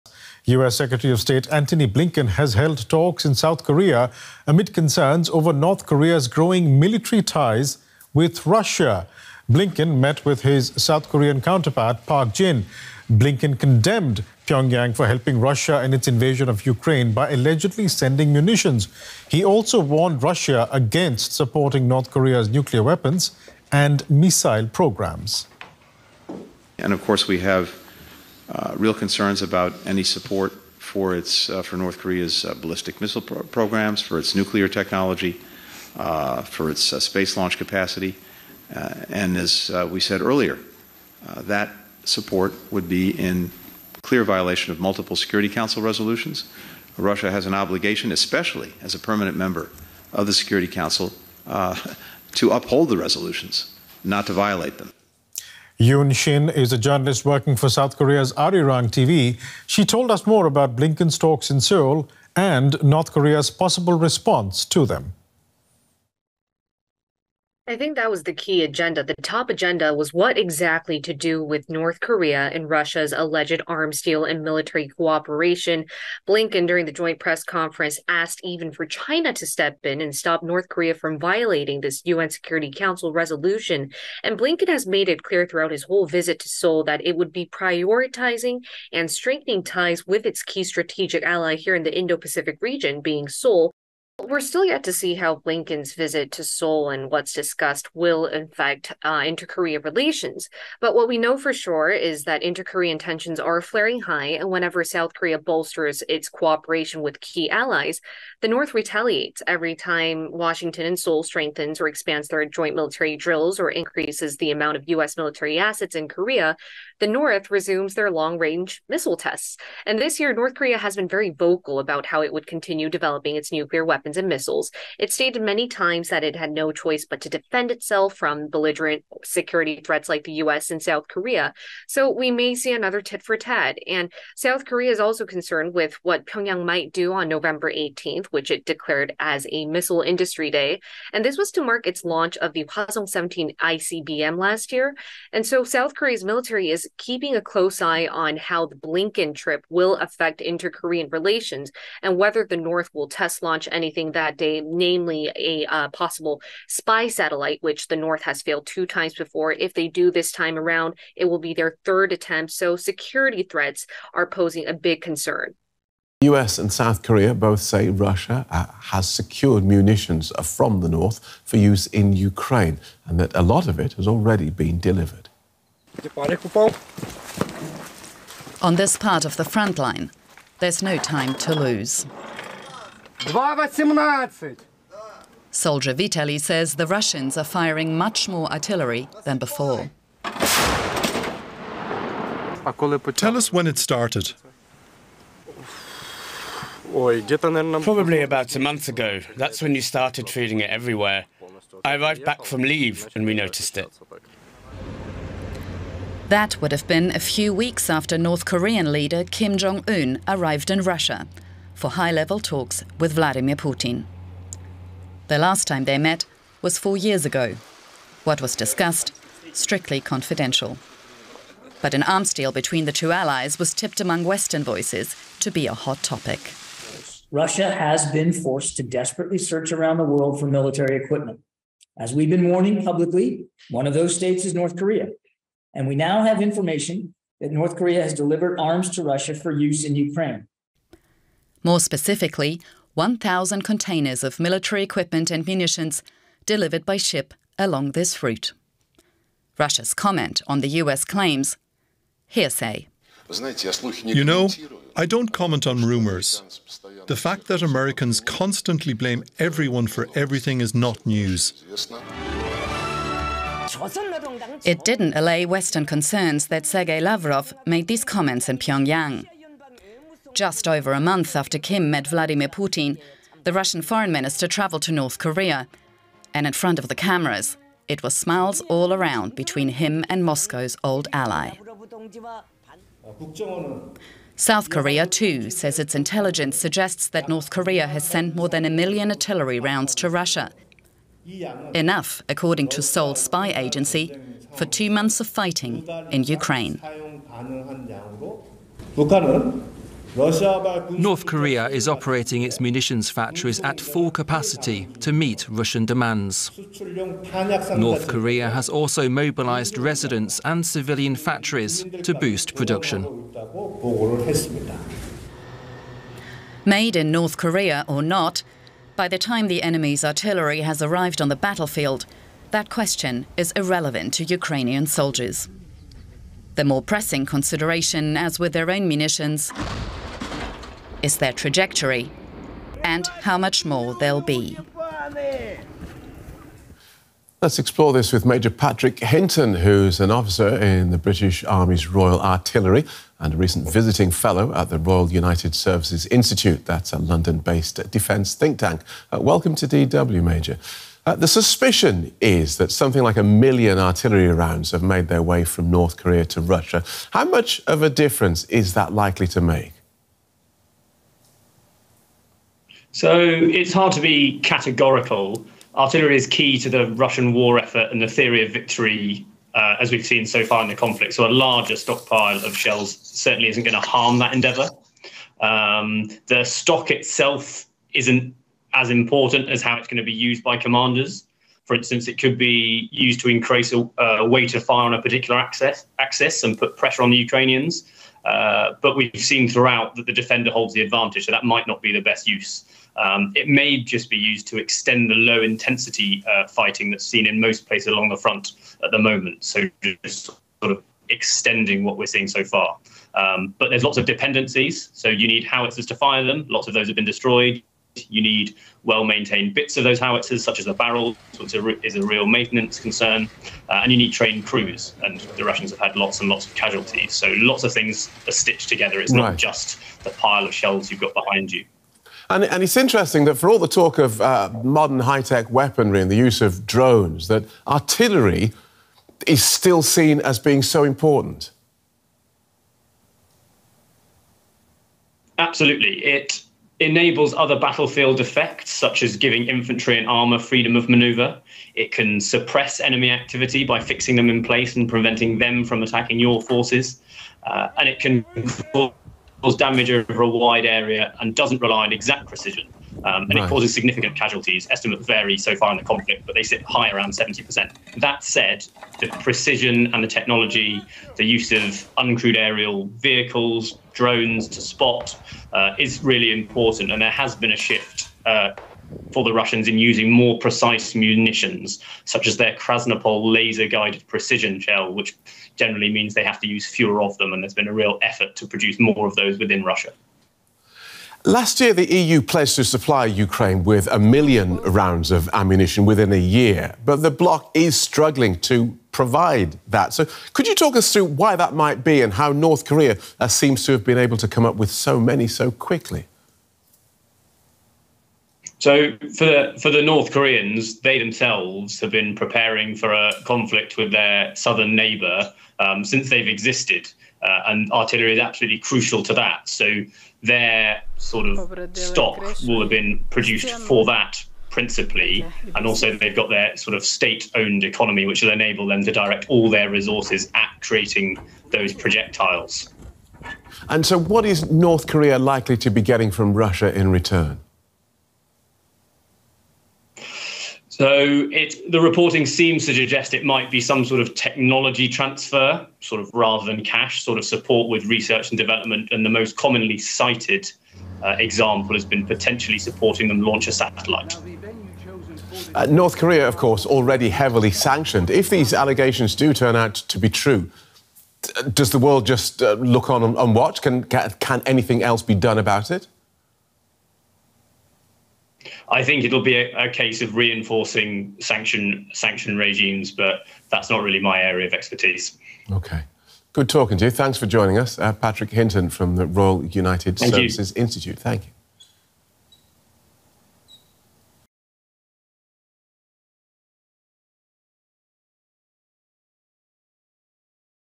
Why the US is convinced that has secured munitions from Pyongyang | DW News
US Secretary of State Antony Blinken has held talks in South Korea amid concerns over North Korea's growing military ties with Russia. Blinken met with his South Korean counterpart Park Jin. Blinken condemned Pyongyang for helping Russia in its invasion of Ukraine by allegedly sending munitions. He also warned Russia against supporting North Korea's nuclear weapons and missile programs.